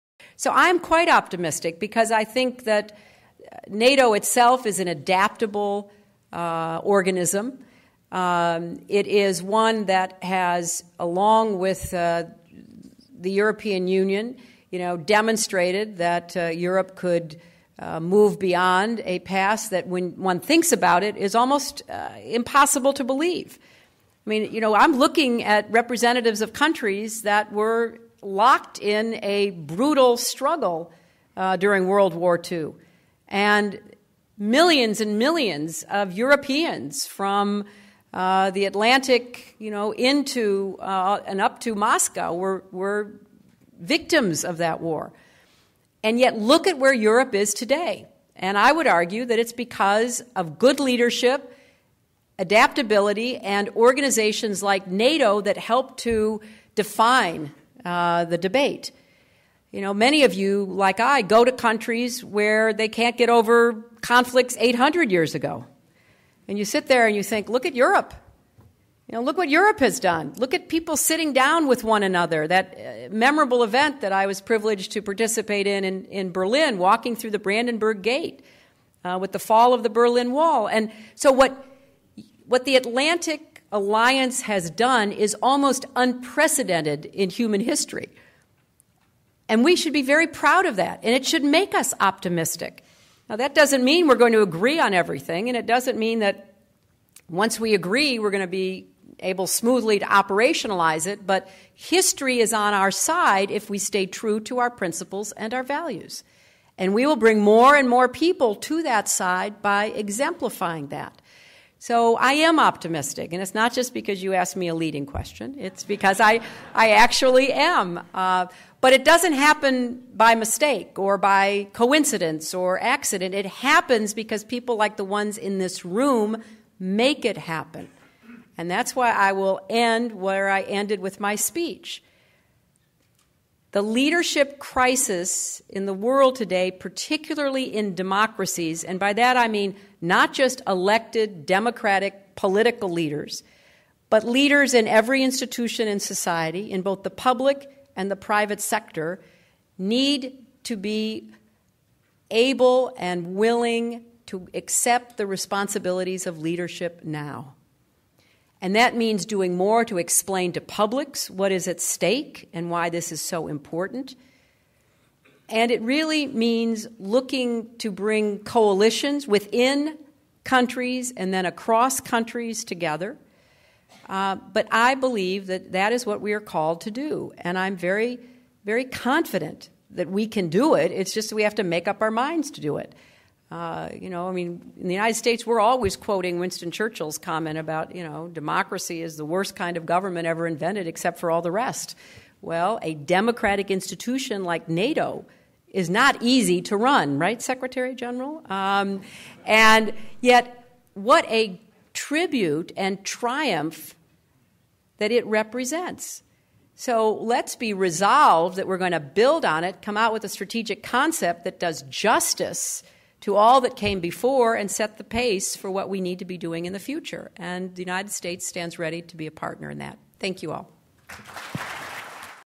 Hillary Clinton on the Leadership Crisis among Democracies
Secretary of State Hillary Clinton points to a leadership crisis among democracies before the Atlantic Council and the partners of the Washington Strategic Concept Seminar. February 22, 2010.